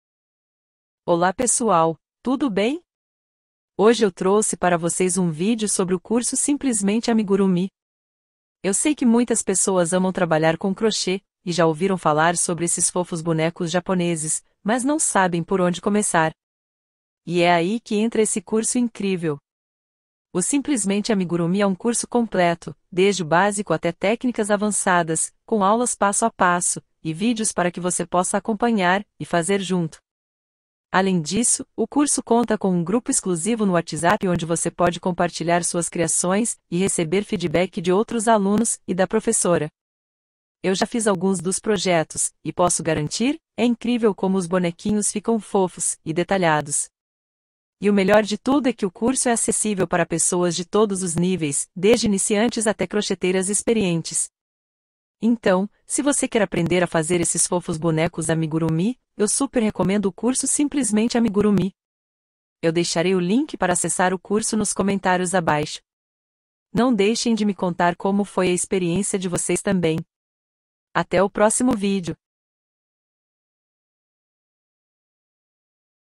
Não consegue Criar Amigurumis? Aprenda Agora de forma Simples com o Curso Simplesmente Amigurumi
O curso "Simplesmente Amigurumi" é um curso online otimizado que ensina como criar bonecos de crochê em estilo japonês, conhecidos como Amigurumis. O curso é ministrado por uma professora experiente em crochê, que compartilha suas técnicas e conhecimentos para que os alunos possam criar seus próprios amigurumis incríveis.

O curso é dividido em módulos e começa com os fundamentos básicos do crochê, para que mesmo quem não tem experiência prévia possa acompanhar. Em seguida, o curso apresenta as técnicas específicas para criar os amigurumis, como a escolha dos fios, agulhas e enchimento, além de ensinar a fazer os pontos e os acabamentos.

Os alunos também terão acesso a uma série de padrões exclusivos de amigurumis, com passo a passo detalhado para criar cada um dos personagens. Além disso, o curso oferece suporte para dúvidas e um grupo exclusivo para interação entre os alunos, possibilitando a troca de experiências e a evolução conjunta na técnica.

Ao final do curso, os alunos terão adquirido habilidades em crochê e em técnicas específicas para amigurumis, além de terem criado